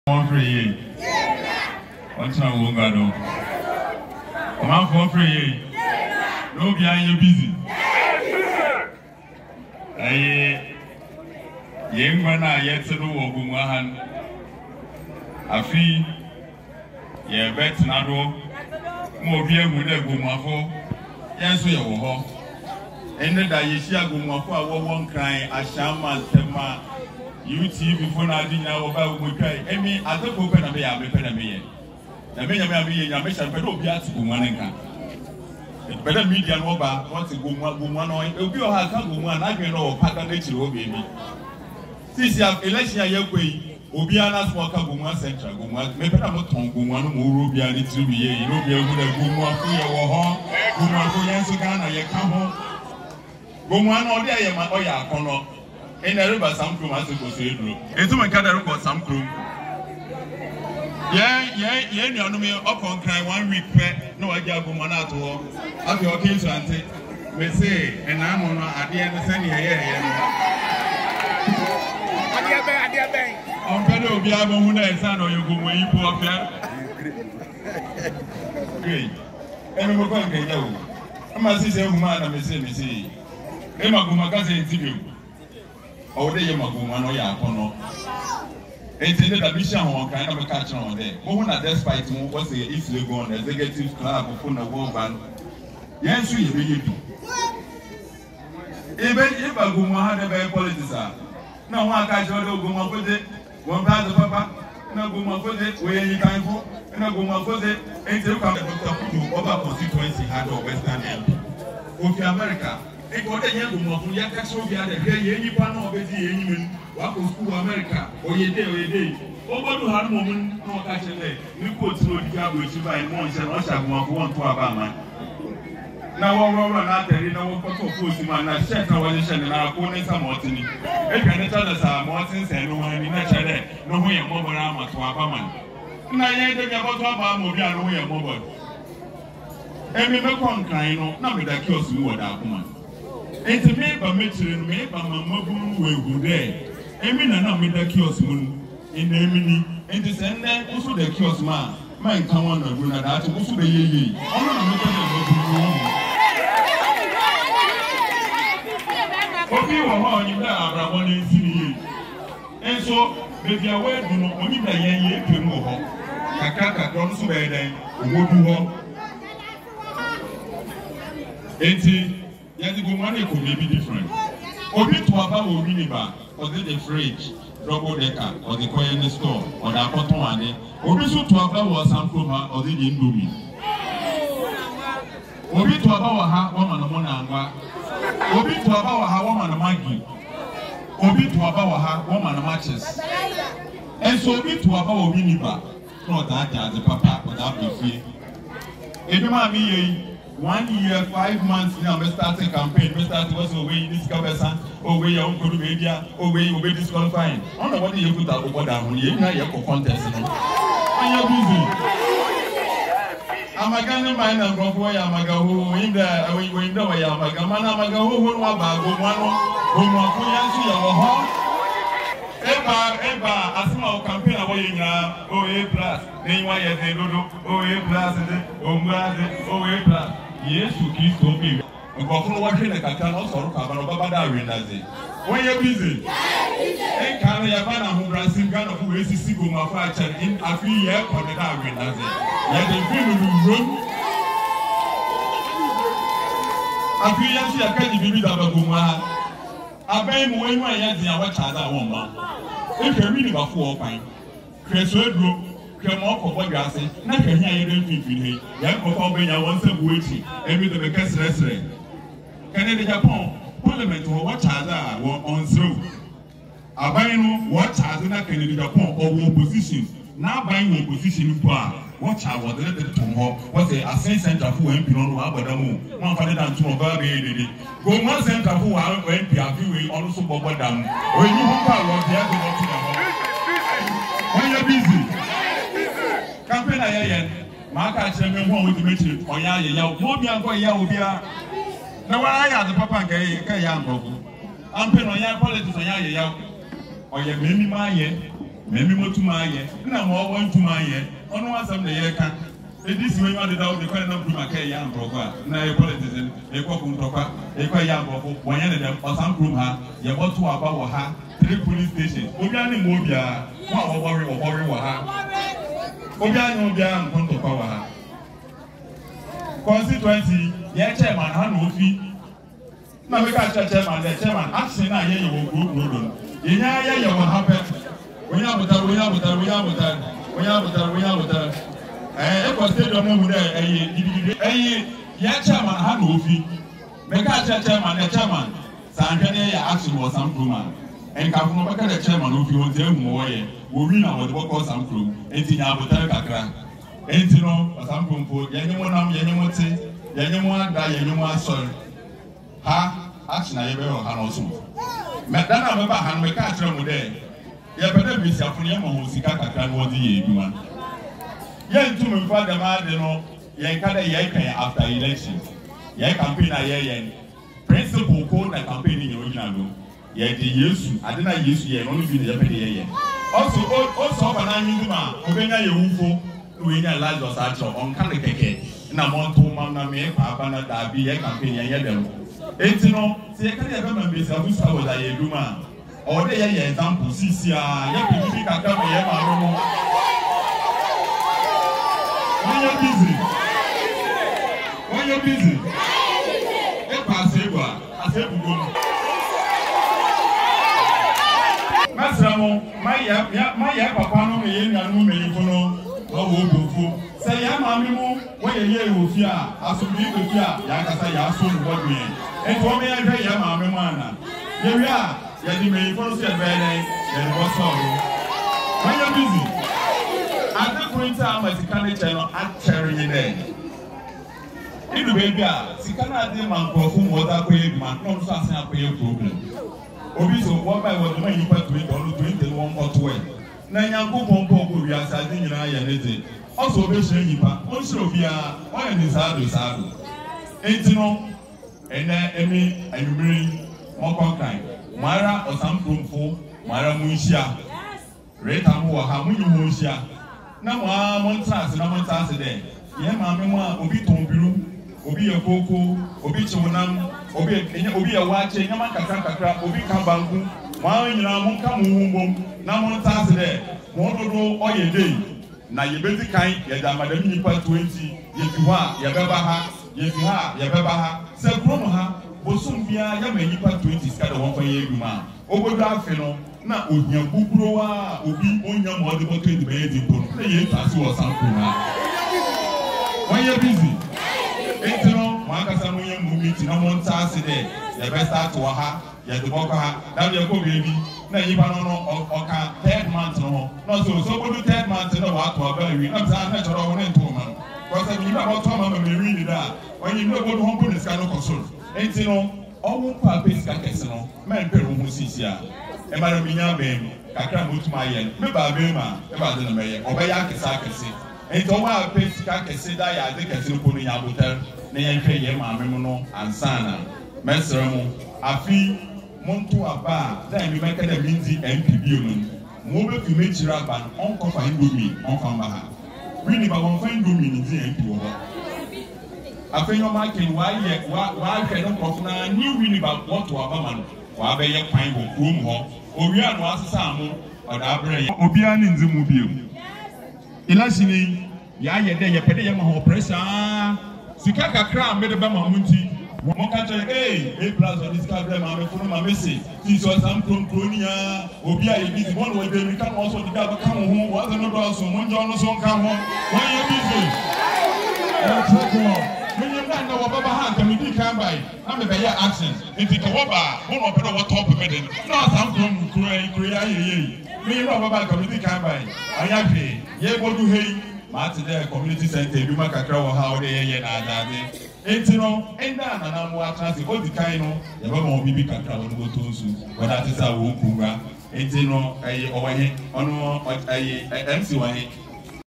A-Plus shakes Gomoa Osamkrom with powerful speech to convince 90% of the people
A-Plus Is an independent candidate in Gomoa Central